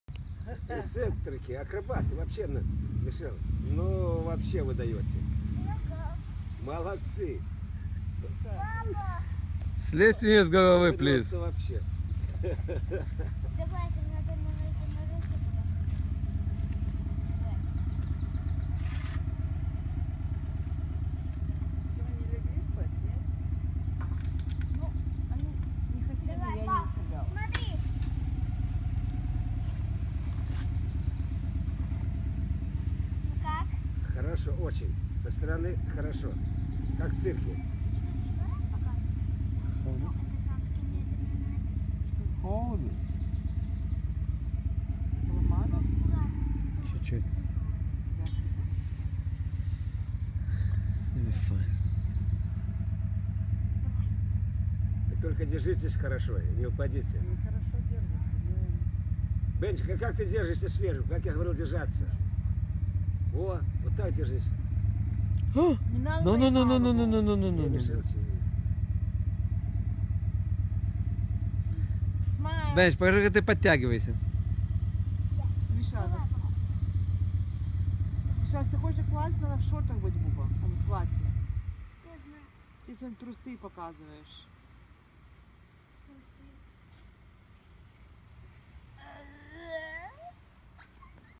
Michelle and Benji playing on the Dome Climber
PA Trails End summer 2016